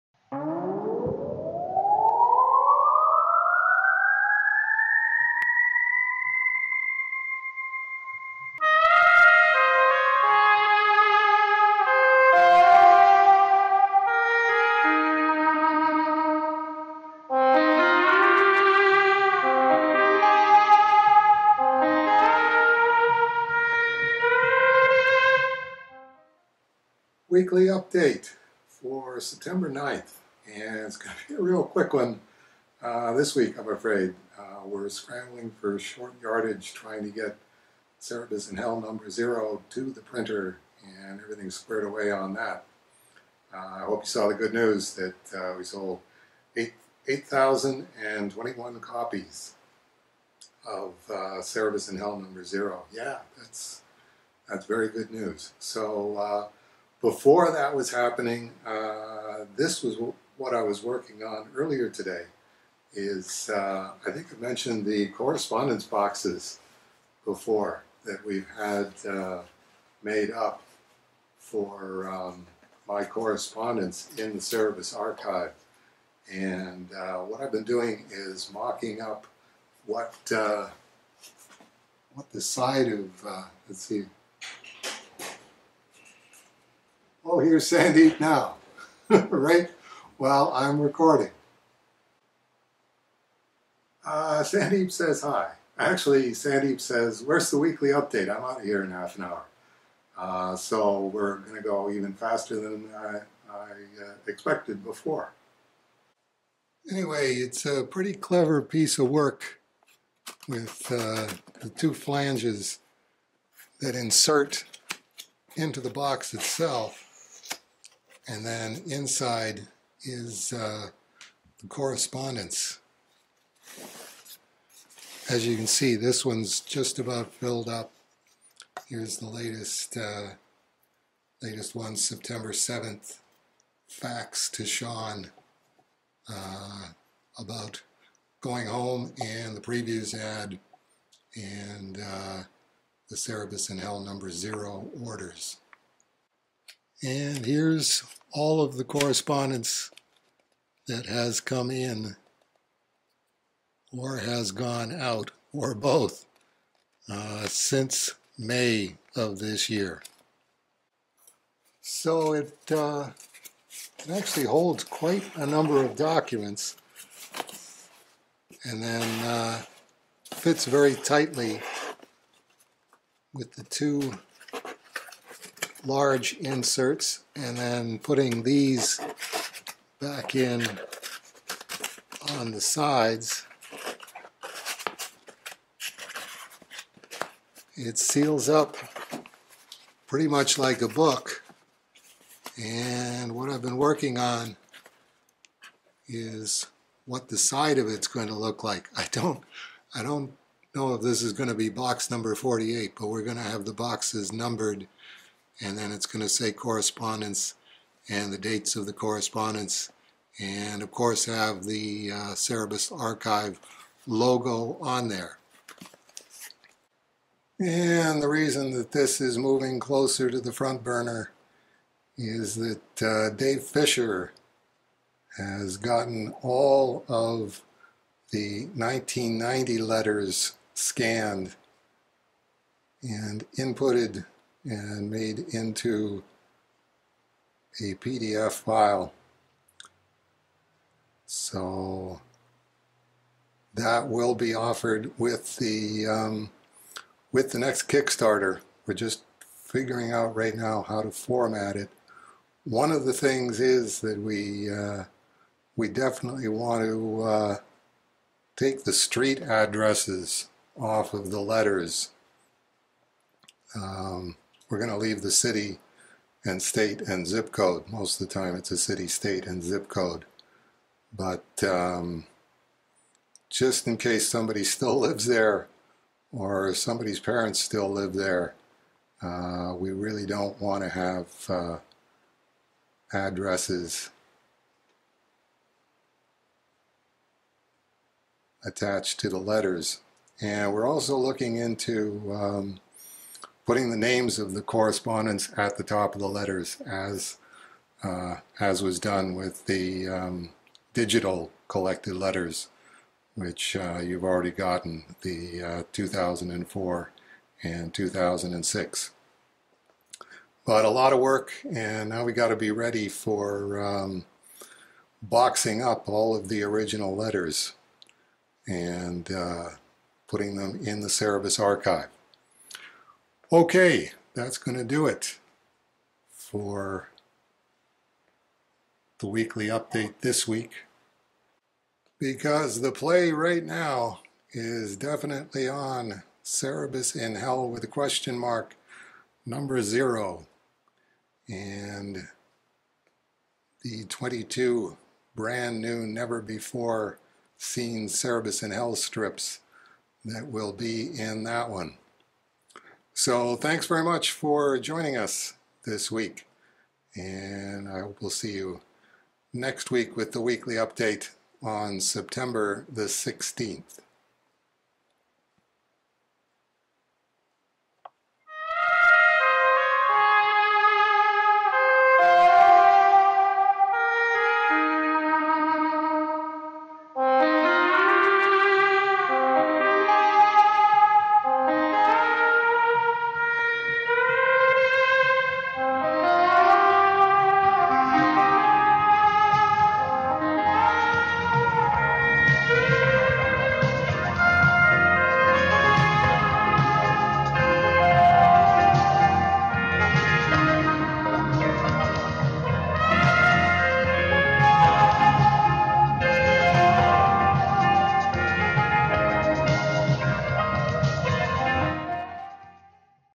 Coming Soon to a Comic Book Store Near You! (Dave's Weekly Update #147)
Dave gets his correspondence organized PLUS a sneak peak at Cerebus In Hell? #0, coming soon to a comic book store near you!